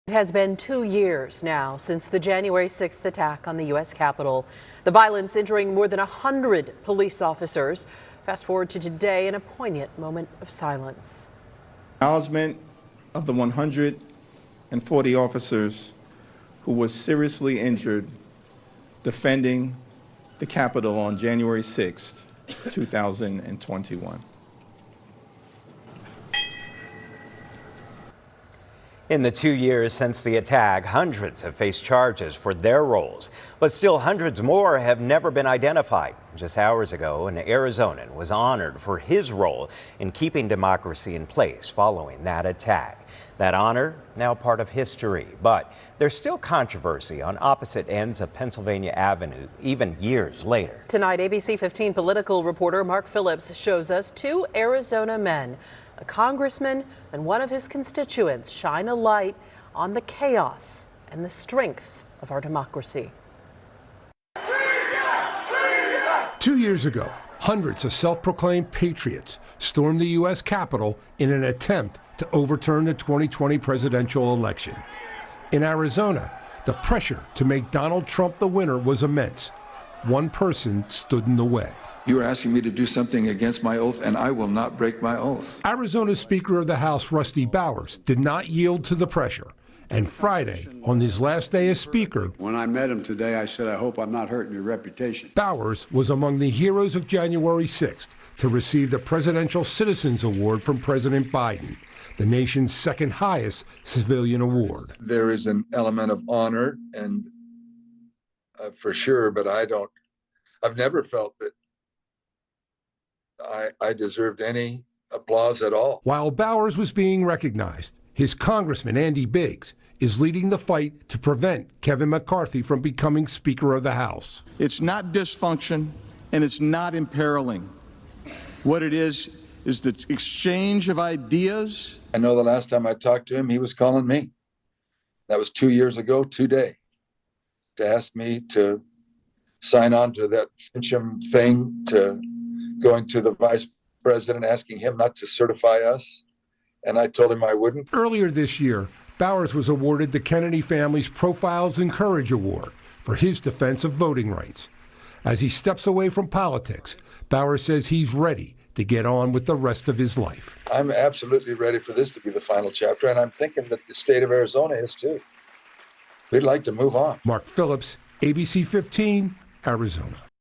Former Arizona House Speaker Rusty Bowers honored with Presidential Citizens Medal
Arizona's Rusty Bowers was awarded the Presidential Citizens Medal two years after the January 6 attack on the U.S. Capitol. Bowers was the Arizona House Speaker during the 2020 election and "(resisted) pressure from those seeking to overturn the election results,” according to the White House.